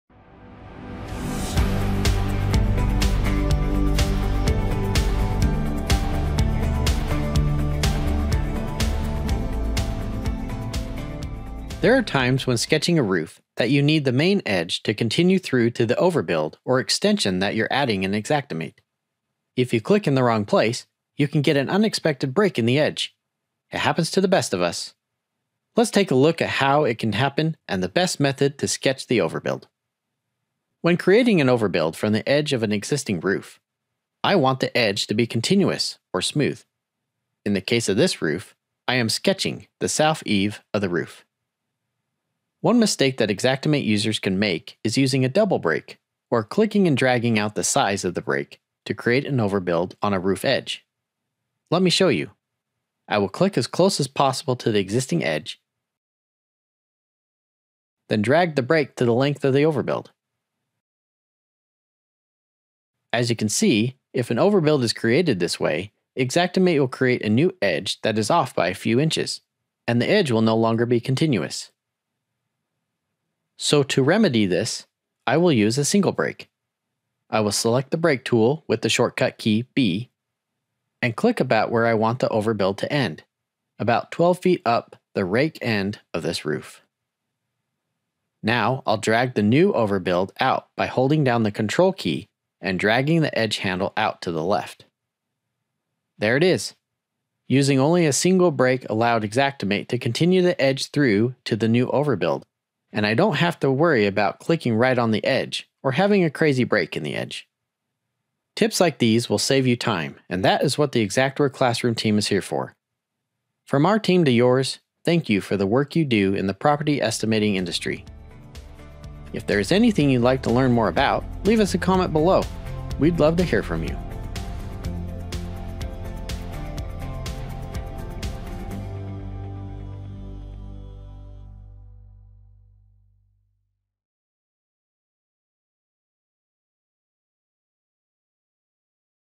Xactimate Xpert Tip: Creating a Continuous Edge While Sketching Roofs
In this Xpert Tip, you will learn how to create a continuous edge between the main roof and an overbuild.

0:10 – Intro
0:30 – Creating an Overbuild
0:43 – Double-Break Mistake
1:13 – Single-Break to create a smooth edge
1:48 – Thanks!

Want to see other topics or have feedback? Let us know

Check out our Construction Basics and Estimation video library - an on-the-job library you can use to learn the basics of construction and how to estimate property damage using Xactimate